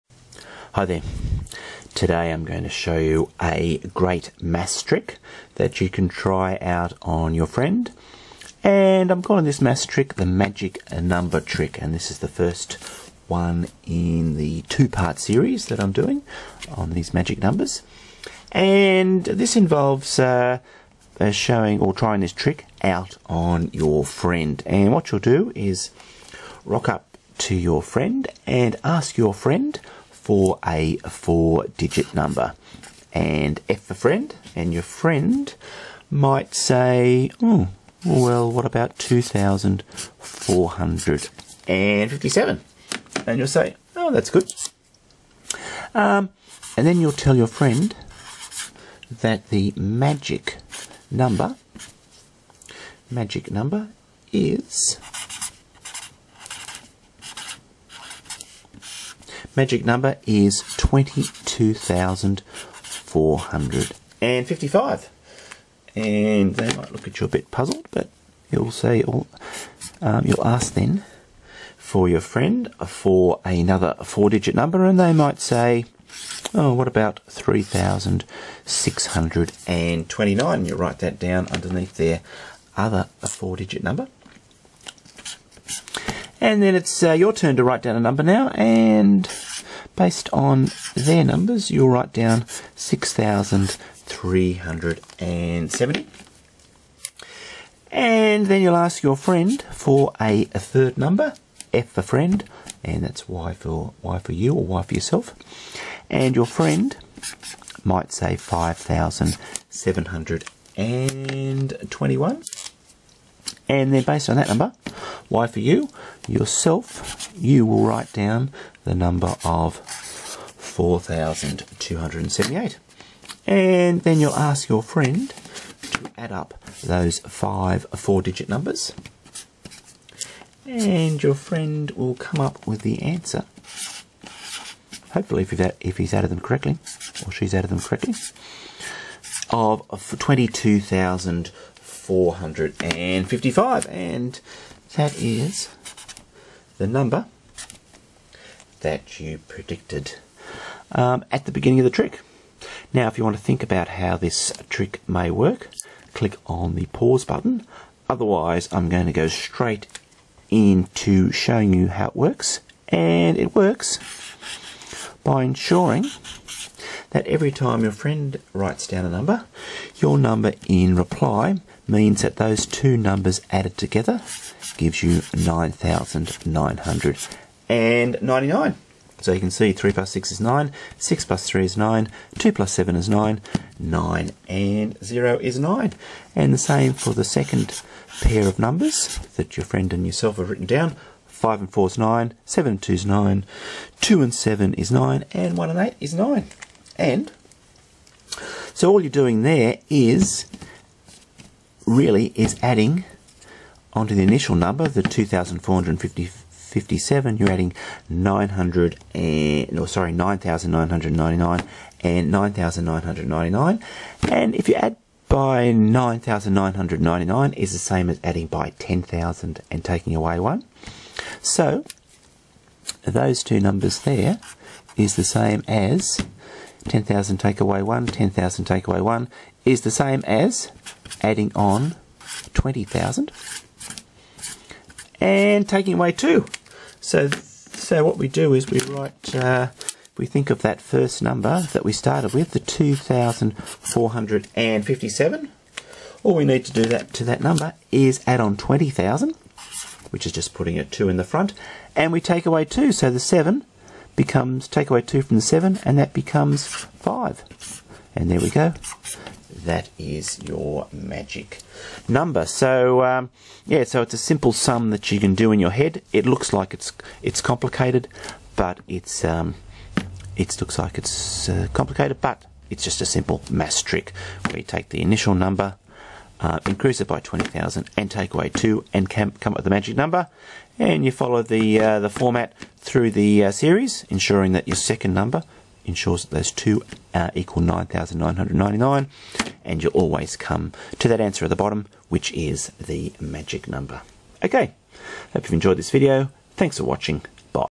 How to do a Simple Math Trick- Know the Answer Before You're Given the Numbers- Magic Number Trick 1
This is a great number trick where you supply the total of 5 numbers before your friend has even given you all the numbers. Watch the video for all the details.  I'm calling the magic number the total of these five numbers.